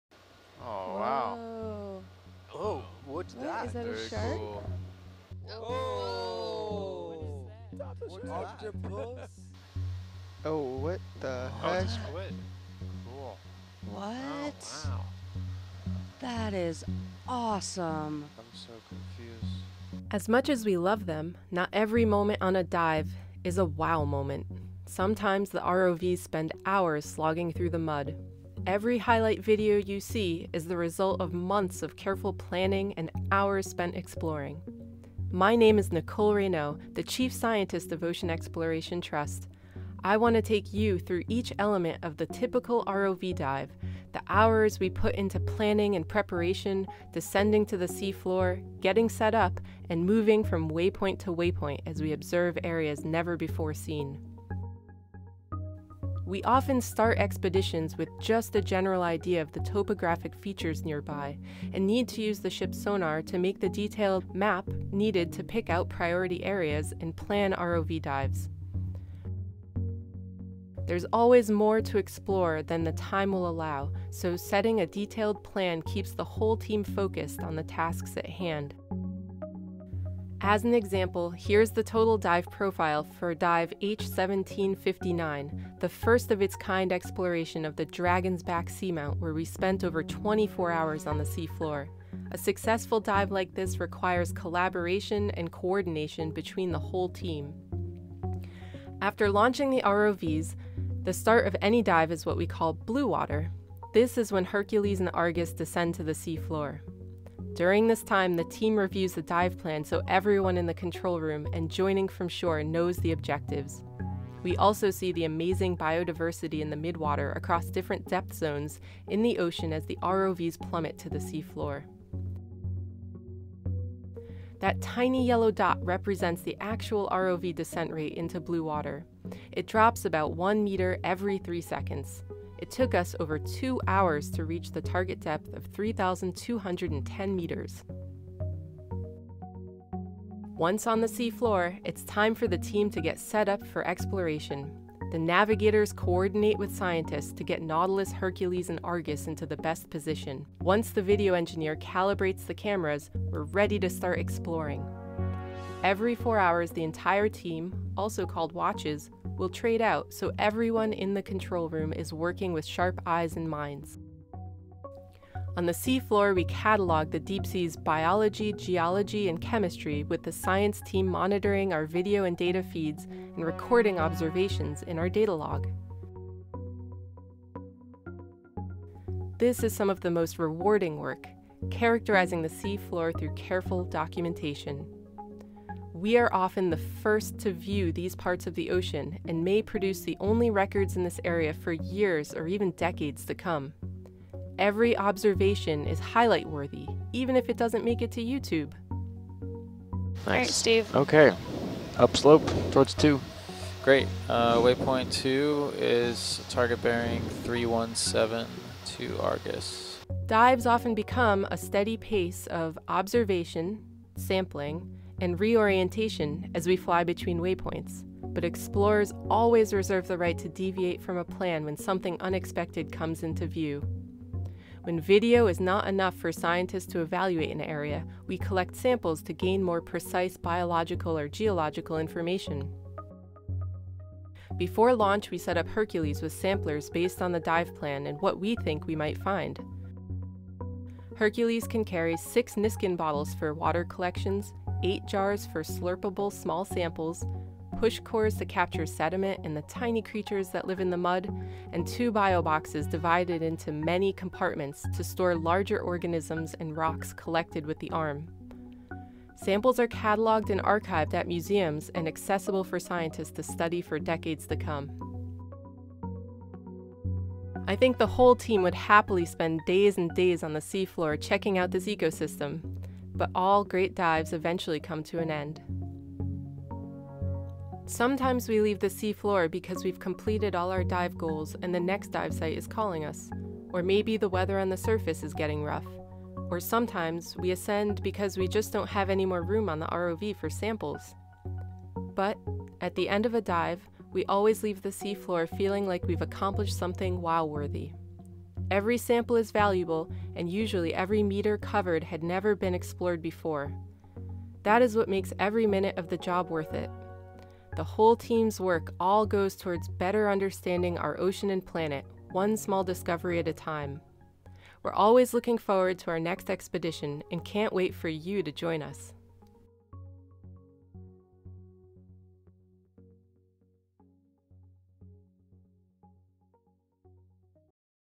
Beyond the Wow: Anatomy of a Dive
Exploring the deep sea with remotely operated vehicles (ROVs) isn’t just about the “WOW” moments when we see eye-to-eye with a sperm whale or marvel at an umbrella-like dumbo octopus. Go behind-the-scenes of an ROV dive with our Chief Scientist, Dr. Nicole Raineault, as she walks through the phases of every dive we conduct: plan, descend, explore, sample, and ascend.

Plan: review seafloor maps and identify target sites of interest to explore
Descend: confirm dive plan as ROVs Hercules and Argus travel down to the seafloor
Explore: move from waypoint to waypoint as we document seafloor unseen by human eyes
Sample: collect organisms, rocks, and water samples to bring back to the surface to study
Ascend: complete dive objectives and recover ROVs and samples for processing

Learn more about our science focus and technology aboard E/V Nautilus
Build your own neutrally buoyant model to match the challenge of an ROV flying over the seafloor